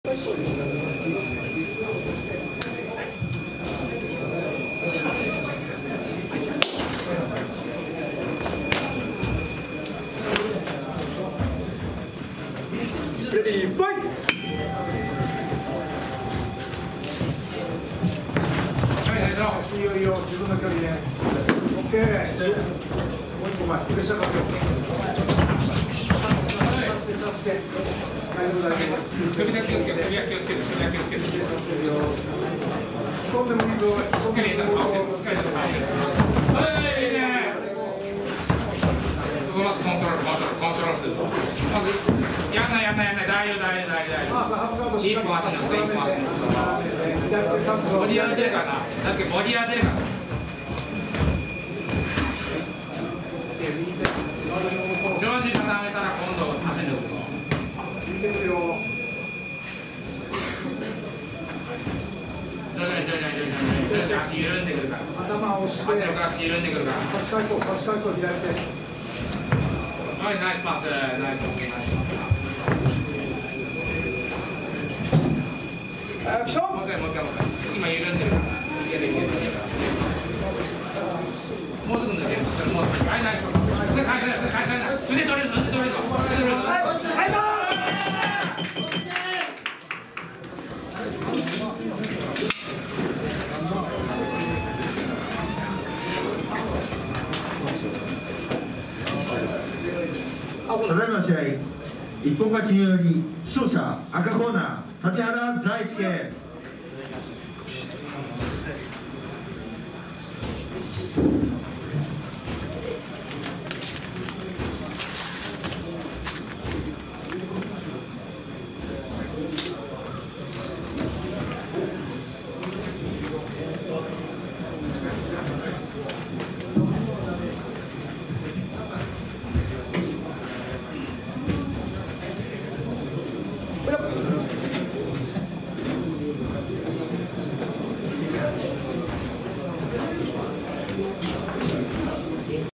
【AMMAC#7】 立原大介（U.W.M）vs 鈴木笙太（総合格闘技ジムmove）
【AMMAC#7】
カテゴリーA　フライ級　3分2R
立原大介（U.W.M）vs　鈴木笙太（総合格闘技ジムmove）

【大会名】アマチュアMMAコンペティション#7

【場所】リバーサルジム東京スタンドアウト　田町芝浦スタジオ

【主催】合同会社SAMANT

【協力】一般社団法人日本MMA審判機構（JMOC）